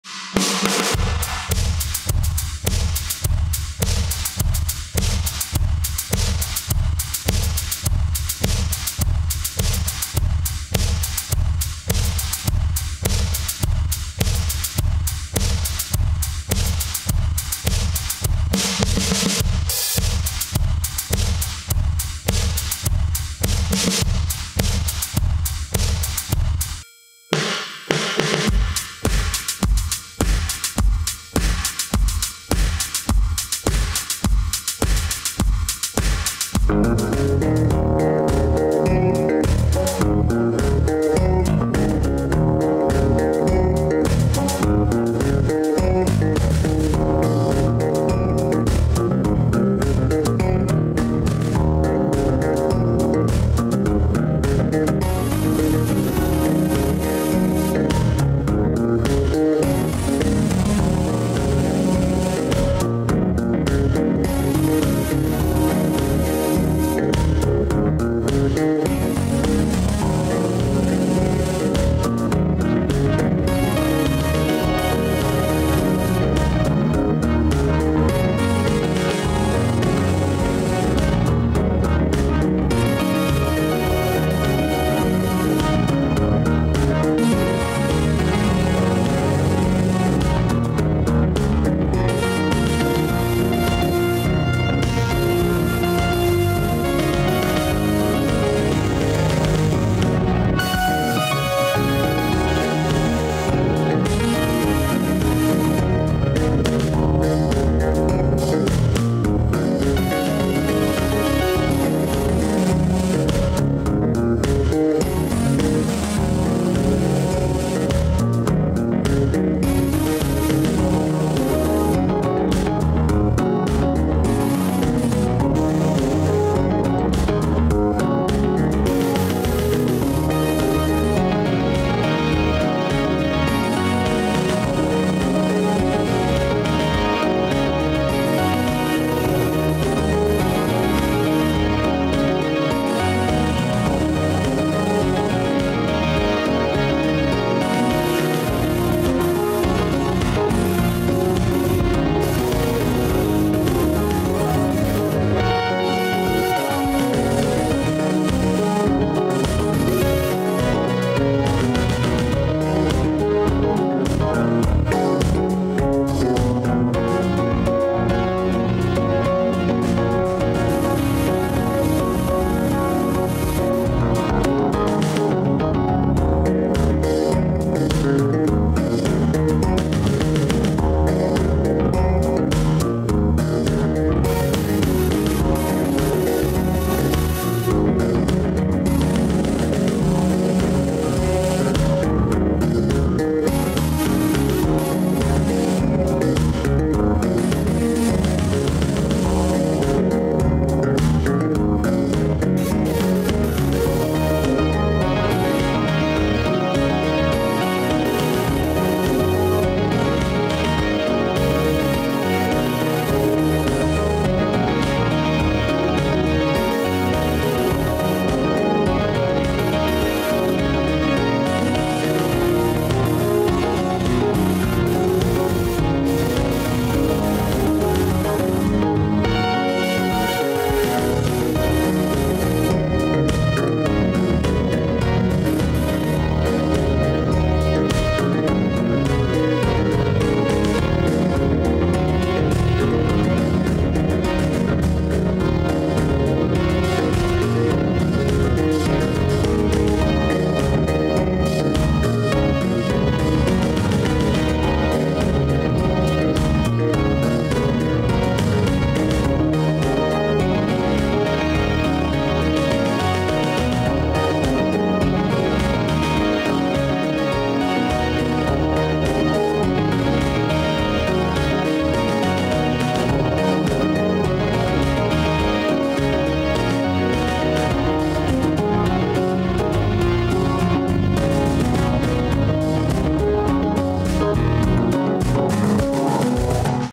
Vol.62【The Making Diorama】Wagon Railway. 和琴鉄道のジオラマ作成
This Movie Show The Making of  Modular Railroad Dioramas.
Railway models of Japanese super express trains and conventional lines.
モジュール式鉄道ジオラマの作成を紹介します。
ヤードの増設をします。
第61弾です。